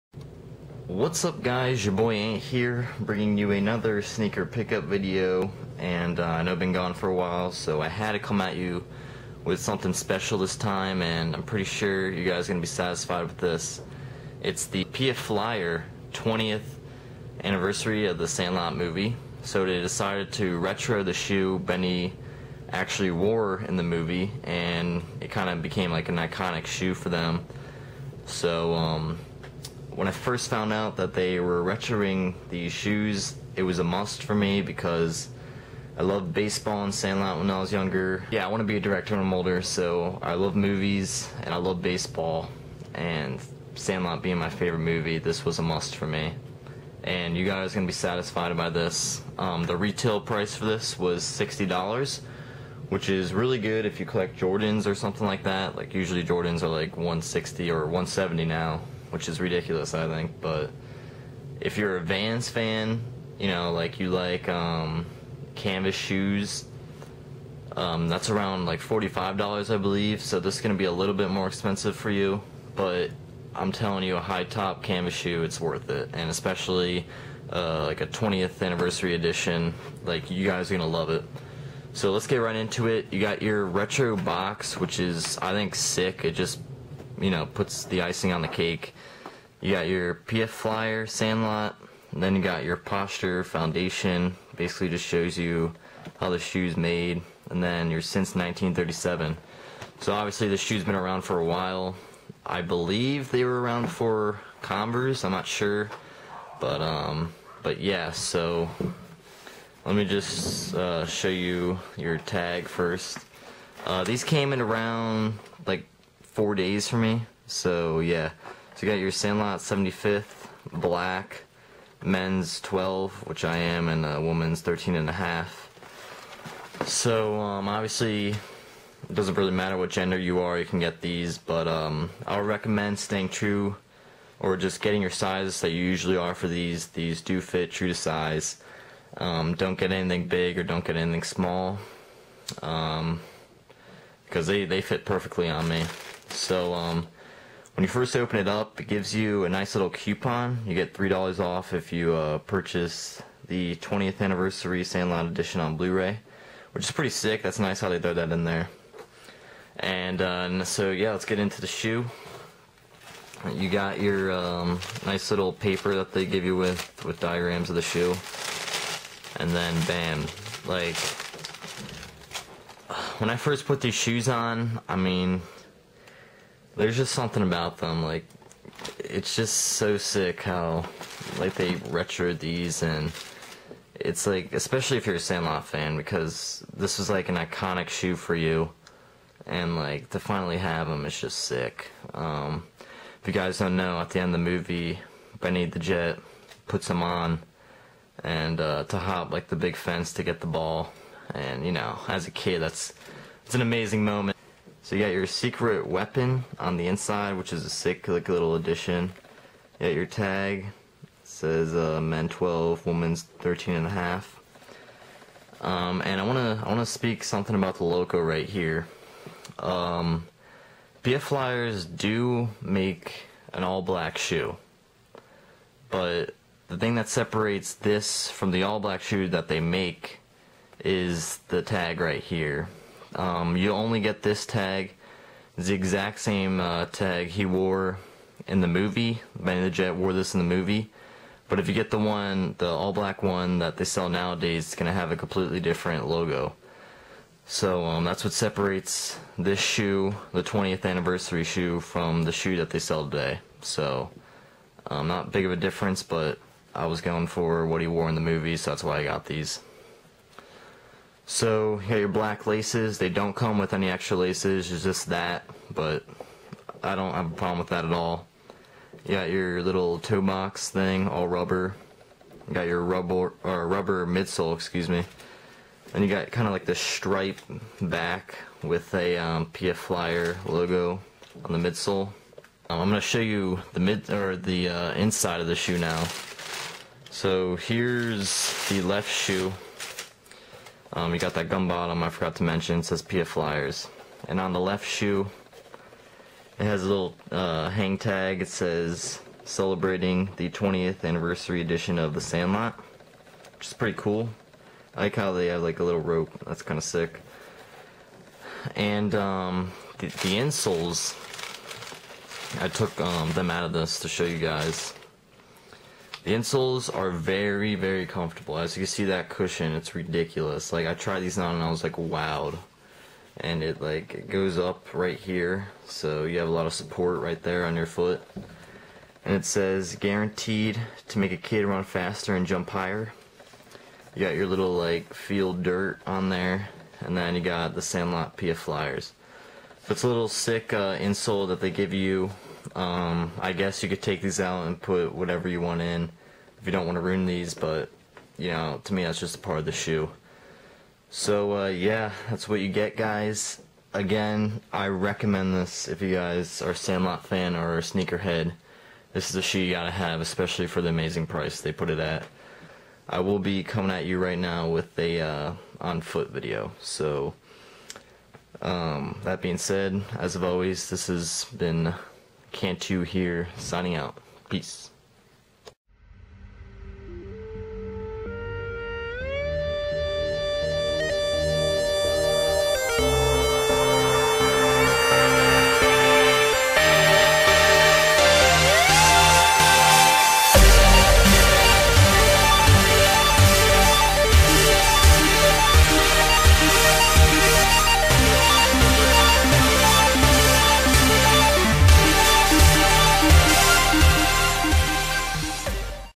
PF Flyer 20th Anniversary Sandlot Shoe
These are a great shoe to have in your collection! Its a must have with a price at $59.99 and with its iconic background. They fit true to size and has a canvas upper with a rubber mid-sole. Sorry about the lighting in the on foot part,  but this wont be the last time you see these shoes on my channel :)